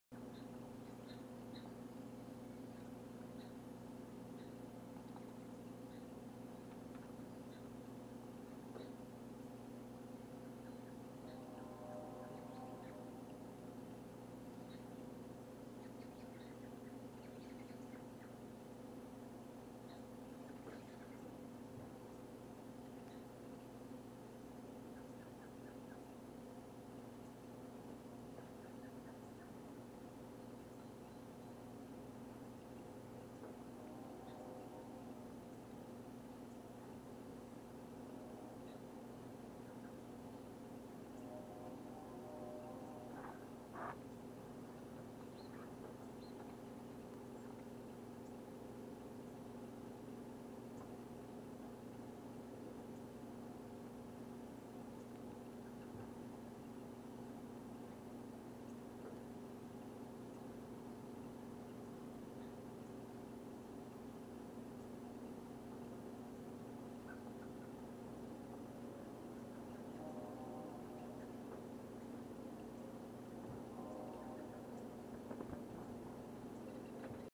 Mrs Carolina Wren vs fresh live mealworms
One half of the resident Carolina Wren pair enjoying some mealworms